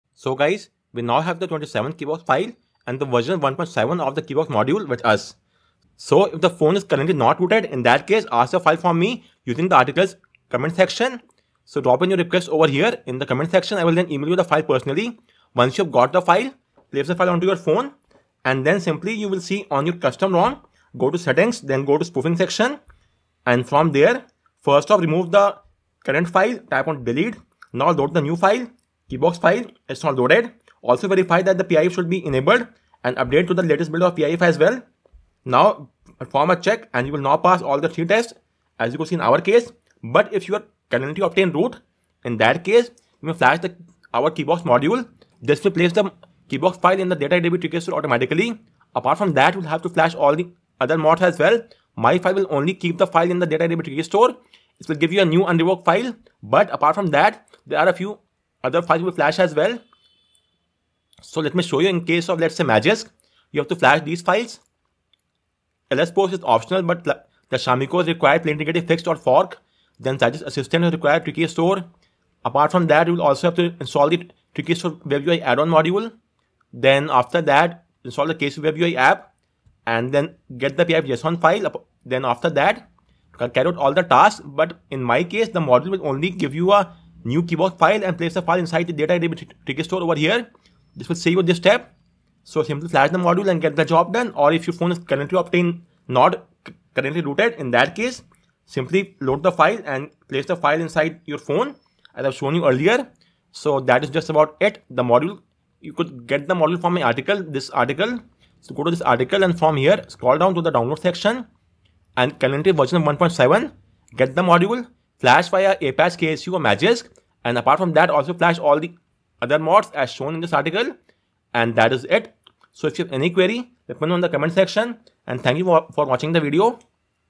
New Keybox File [27th] and Keybox Module [v1.7] are Now Live
We now have the latest [27th] Keybox XML file that will help you pass the Strong Integrity Test [apart from Device and Basic]. Likewise, the updated version of Droidwin Keybox Module [v1.7] is also live.

"Cherish each remaining keybox, because there aren't many left!"